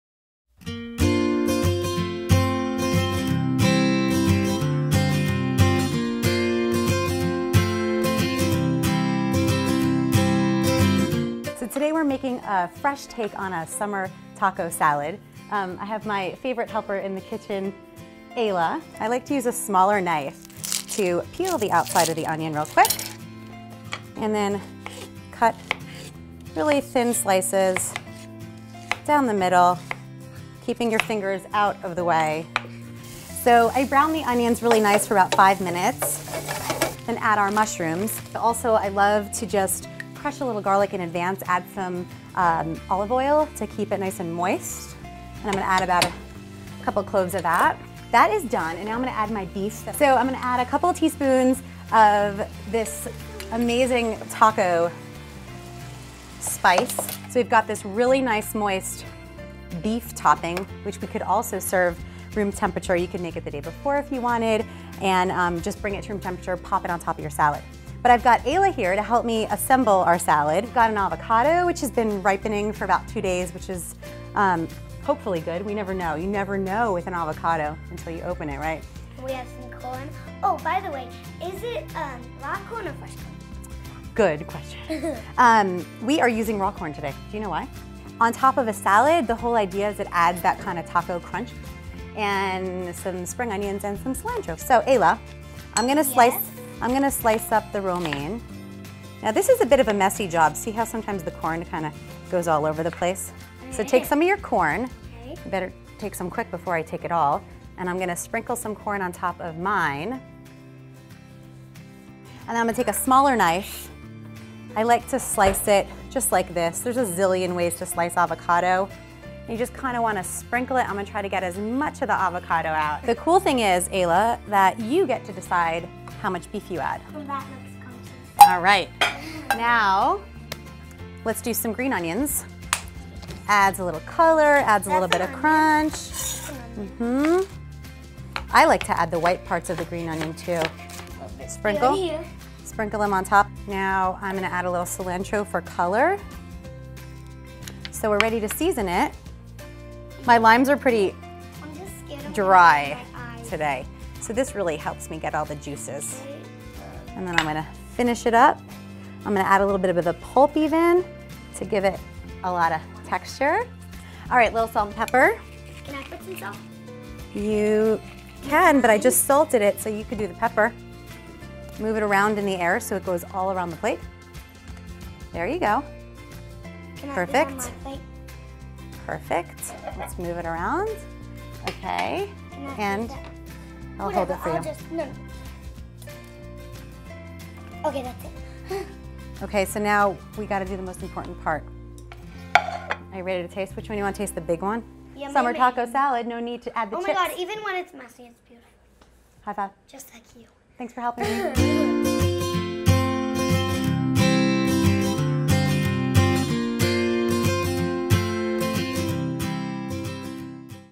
The Flexible Chef | Summer Taco Salad Recipe
Say goodbye to highly processed tacos and hello to this healthy summer taco salad recipe by The Flexible Chef's Nealy Fischer. This recipe requires sautéing beef and vegetables. Then, topping the delicious mixture on a bed of fresh veggies. Not only is it healthy, this taco salad is delicious (even kids approve)!

Get More Healthy Recipes by founder Nealy Fischer by following her on social: